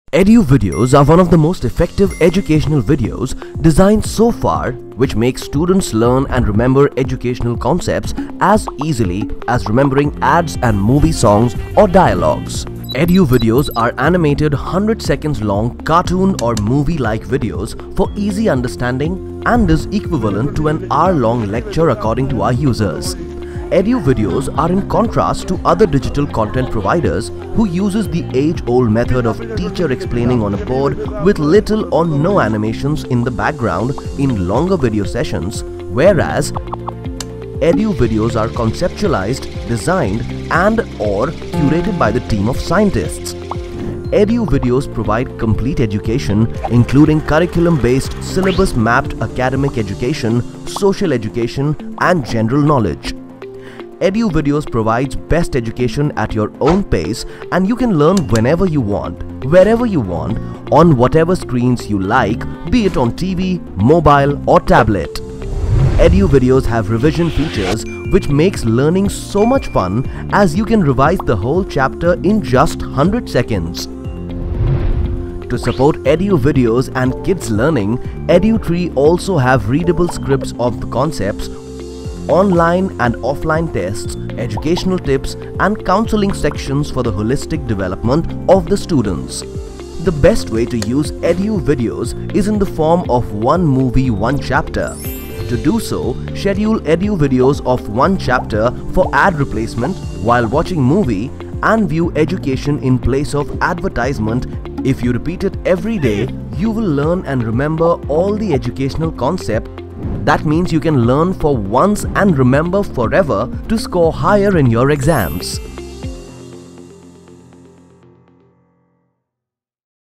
EduVideos | Fun Learning Videos Curated by Scientists | Cerebroz EduTree
EduVideos are syllabus mapped, 2 minutes long educational videos designed by the team of scientist and educators to make learning as simple as possible. In addition to curriculum based content, EduVideos are also available for general knowledge (GK) making sure of the kids overall growth and development. TV Ads will be replaced by EduVideos, so a child can learn whole chapter while watching one movie on TV. It makes learning fun as much as watching TV and playing games on mobile. Each EduVideo is followed by a test to make sure that child has learned effectiely from videos.